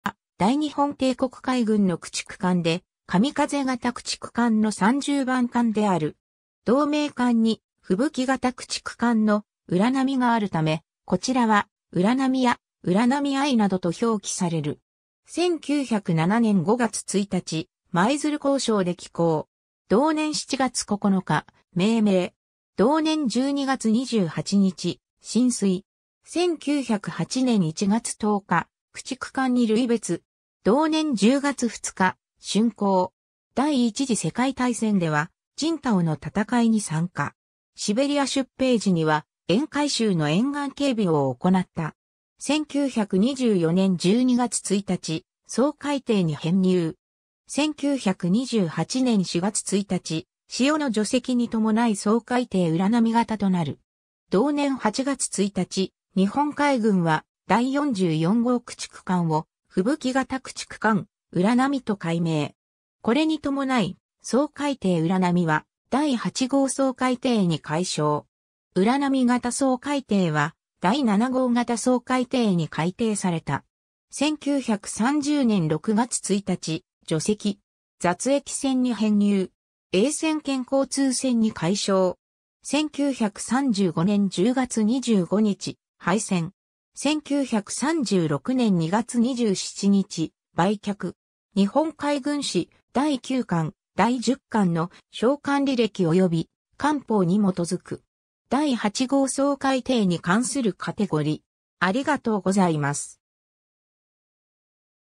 浦波 (初代神風型駆逐艦)
神風型駆逐艦_(初代)
浦波（うらなみ）は、大日本帝国海軍の駆逐艦で、神風型駆逐艦 (初代)の30番艦である。
同名艦に吹雪型駆逐艦（特I型）の「浦波」があるため、こちらは「浦波 (初代)」や「浦波I」などと表記される。
 1907年（明治40年）5月1日、舞鶴工廠で起工。
同年7月9日、命名（製造番号、第30号駆逐艦）。
同年12月28日、進水。
1908年（明治41年）1月10日、駆逐艦に類別。
同年10月2日、竣工。
 第一次世界大戦では、青島の戦いに参加。
シベリア出兵時には沿海州の沿岸警備を行った。
 1924年（大正13年）12月1日、掃海艇に編入。
 1928年（昭和3年）4月1日、「潮」の除籍にともない掃海艇浦波型となる。
同年8月1日、日本海軍は第44号駆逐艦を吹雪型駆逐艦「浦波」と改名。
これにともない「掃海艇 浦波」は第八号掃海艇に改称。
浦波型掃海艇は第七号型掃海艇に改訂された。
1930年（昭和5年）6月1日、除籍。
雑役船に編入。
曳船兼交通船（公称第758号）に改称。
1935年（昭和10年）10月25日、廃船。
1936年（昭和11年）2月27日、売却。
 第八号掃海艇 (初代)に関するカテゴリ: